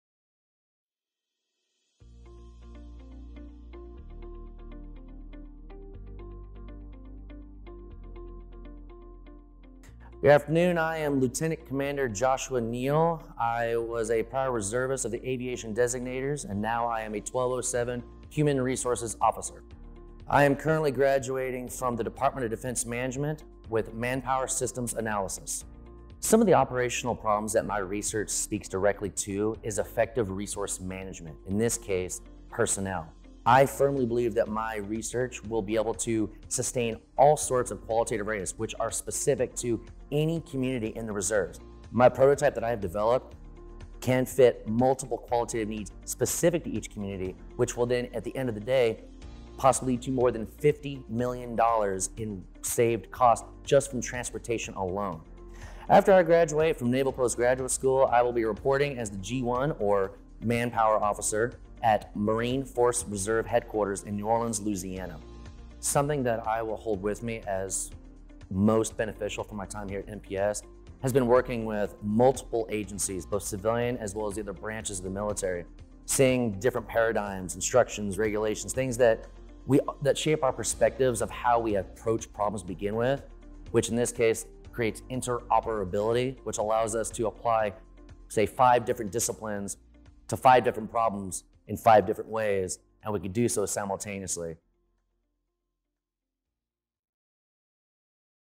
2023 Winter Quarter Graduate Spotlight – Lt. Cmdr. Joshua Neal, U.S. Navy
U.S. Navy Lt. Cmdr. Joshua Neal is Human Resources officer in the Manpower Systems Analysis program. A former reservist, Neal wanted to use his experience in the NavyReserve force to explore opportunities for efficiency and cost savings in the personnel billeting assignment process while retaining warfighting readiness.

Using a custom, mixed method analysis that prioritized qualitative matching between personnel and billets across multiple criteria with no loss to readiness, Neal’s study revealed optimal personnel-to-billets match value across the specific community he studied – but at just 64 percent of the original cost. Ultimately, the prototype developed in Neal’s study could be utilized across the Reserve labor supply, ensuring Reservists bring the critical skills and experience to our strategic warfighting readiness and in alignment with the vision of the Chief of Navy Reserve at a savings of at least $50 million dollars.

Neal is among the 218 warrior-scholars celebrating the completion of their NPS studies during the Winter Quarter Graduation ceremony on Friday, March 24, at 10 a.m. Pacific (1 p.m. Eastern), featuring commencement speaker Vice Adm. John B. Mustin, Chief of Navy Reserve.